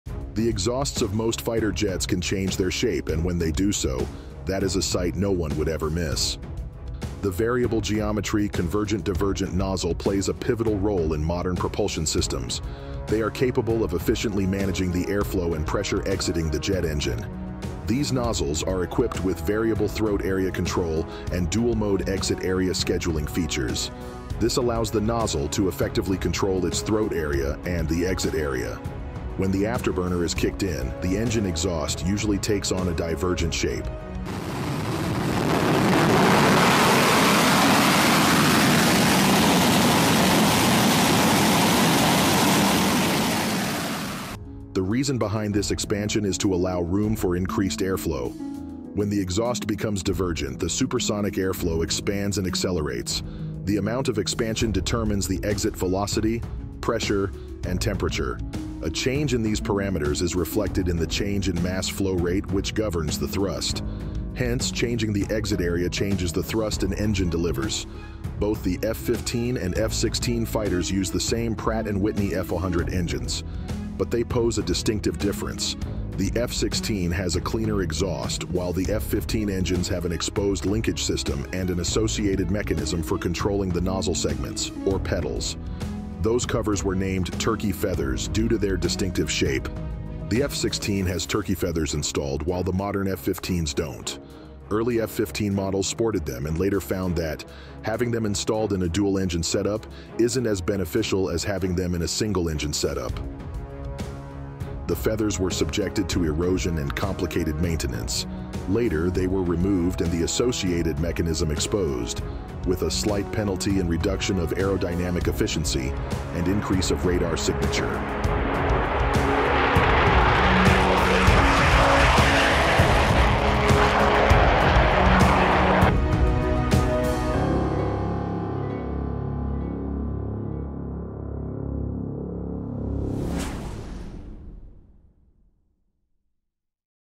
The Mighty Exhausts of the F-15 and F-16 Fighters
The exhausts of most fighter jets can change their shape, and when they do so, that is a sight no one would ever miss.

The variable-geometry Convergent-Divergent (CD) nozzle plays a pivotal role in modern propulsion systems. They are capable of efficiently managing the airflow and pressure exiting the jet engine. These nozzles are equipped with variable throat area control and dual-mode exit area scheduling features. This allows the nozzle to effectively control its throat area and the exit area.

When the afterburner is kicked in, the engine exhaust usually takes on a divergent shape. The reason behind this expansion is to allow room for increased airflow. When the exhaust becomes divergent, the supersonic airflow expands and accelerates. The amount of expansion determines the exit velocity, pressure, and temperature. A change in these parameters is reflected in the change in mass flow rate, which governs the thrust. Hence, changing the exit area changes the thrust an engine delivers.

Both the F-15 and F-16 fighters use the same Pratt and Whitney F100 engines. But they pose a distinctive difference. The F-16 has a cleaner exhaust, while the F-15 engines have an exposed linkage system and an associated mechanism for controlling the nozzle segments, or petals. Those covers were named ‘Turkey Feathers’ due to their distinctive shape. The F-16 has turkey feathers installed, while the modern F-15s don’t. Early F-15 models sported them and later found that having them installed in a dual-engine setup isn’t as beneficial as having them in a single-engine setup. The feathers were subjected to erosion and complicated maintenance. Later, they were removed and the associated mechanism exposed, with a slight penalty in reduction of aerodynamic efficiency and increase of radar signature.